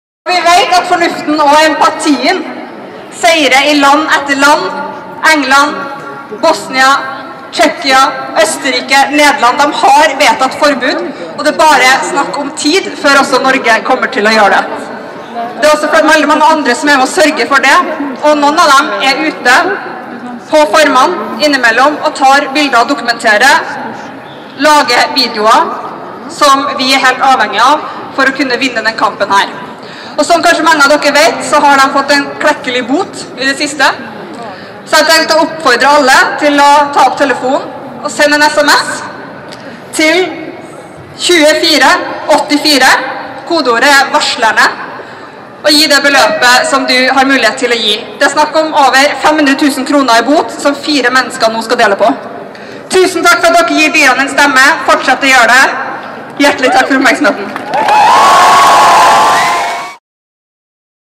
Une Bastholm på fakkeltog mot pels
Takk til Une Bastholm​ som hold en fantastisk appell på NOAHs fakkeltog mot pels i går!
De er utrolig inspirerende å se så mange mennesker stå samlet mot pelsindustrien.
Støtt oss så kampen kan fortsette: Send SMS med kodeord varslerne [beløp] til 2484 eller Vipps til 91883.